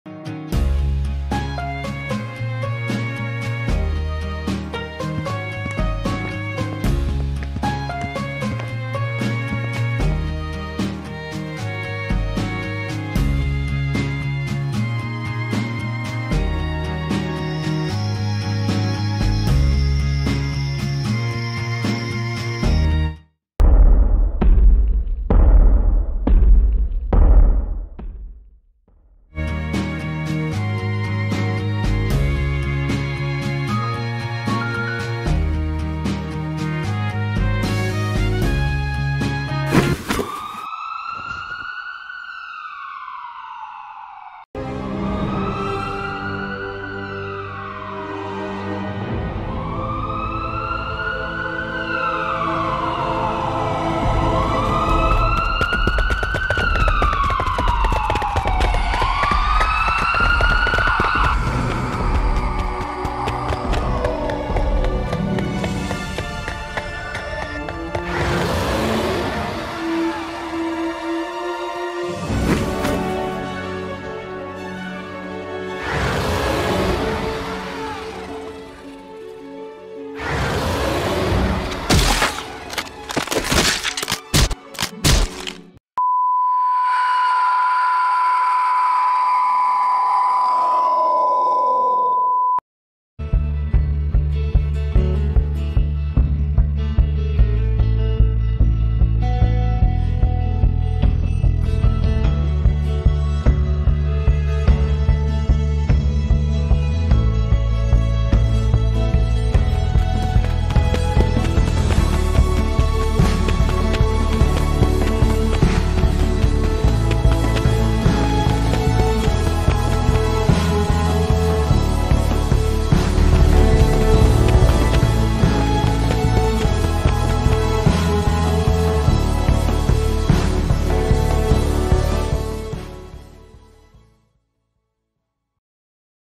AFA SP23: Anthropocene Pilot Episode -- Sydney Greeno
CW: blood/violence

Animatic created for the UC Berkeley Art for Animation Decal, Spring 2023